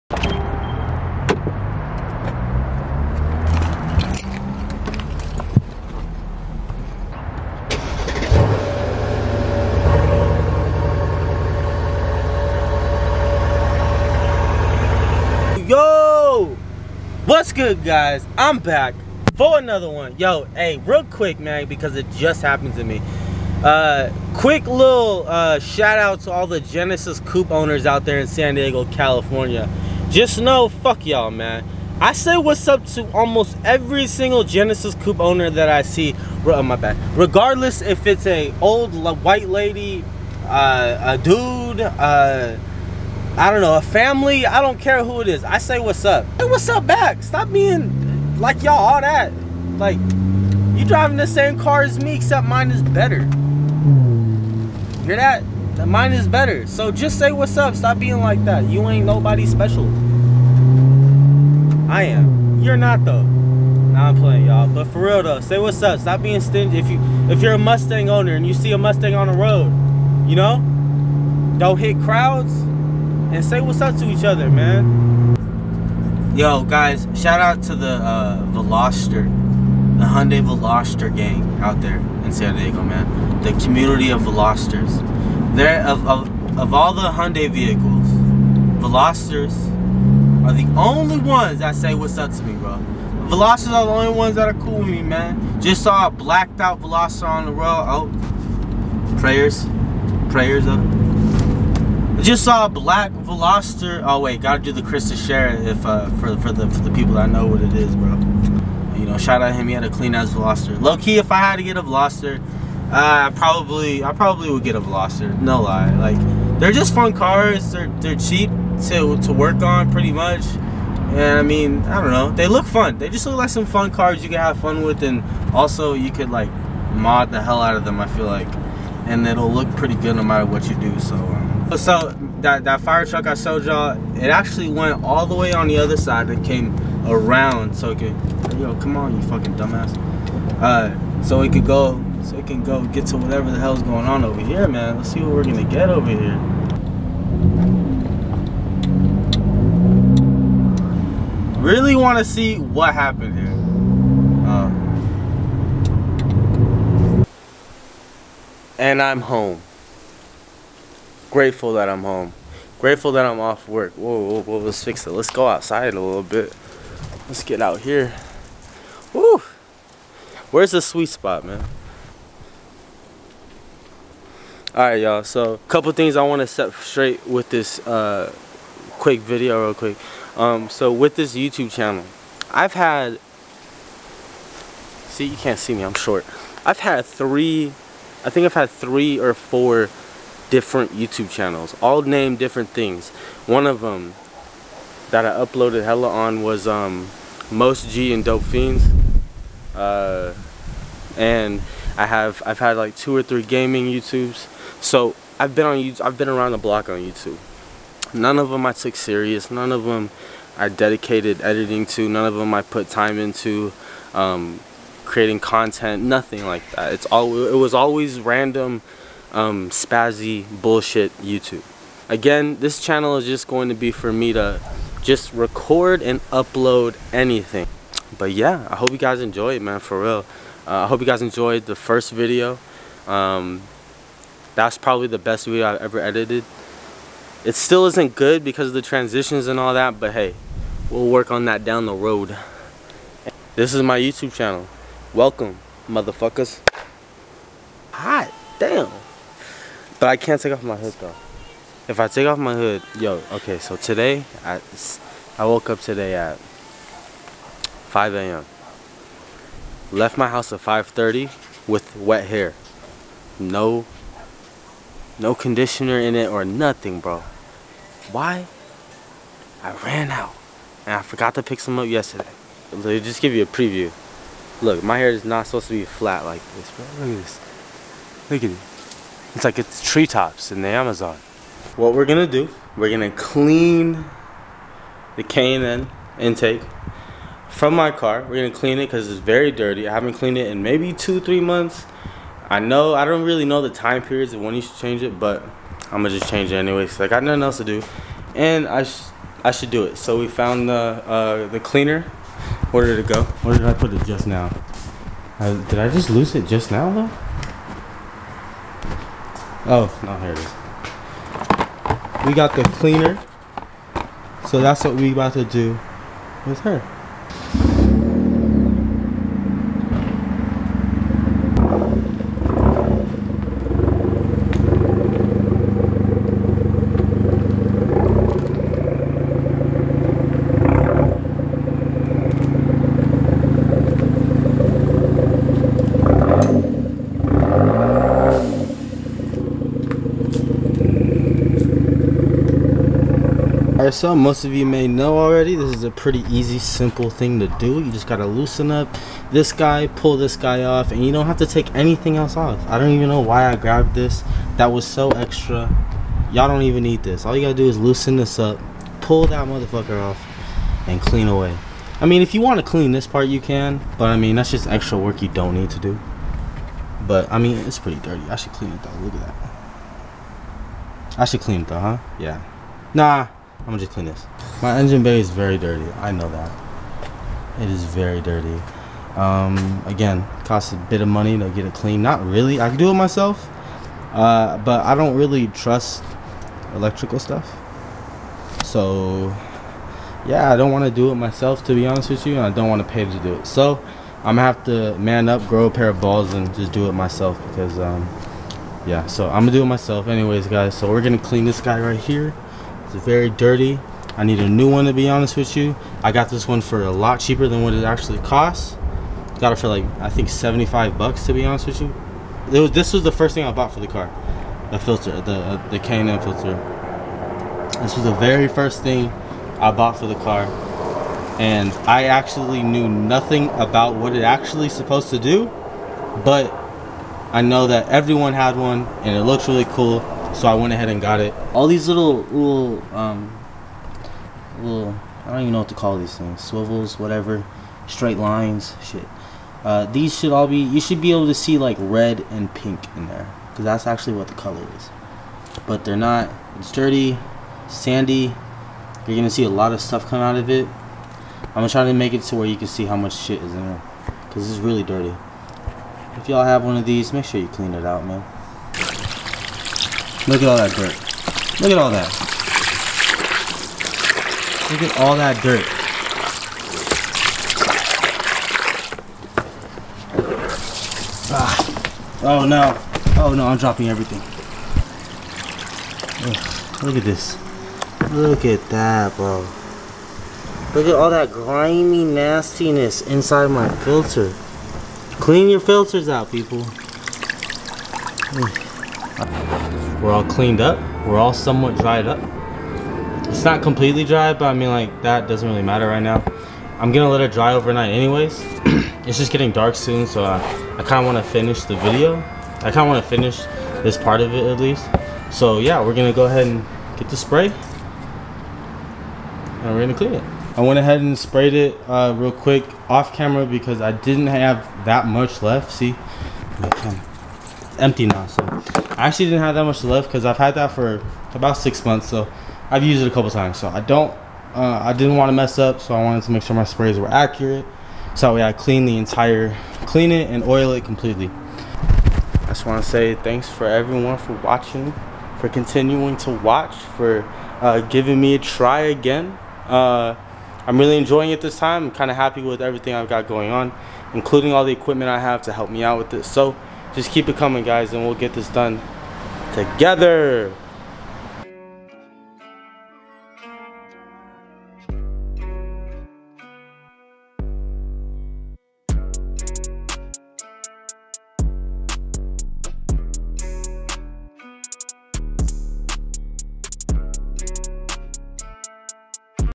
Intake cleaning after 6 Months!!!
Update to my channel and how it will be used! I also clean my K&N Intake after 6 Months!!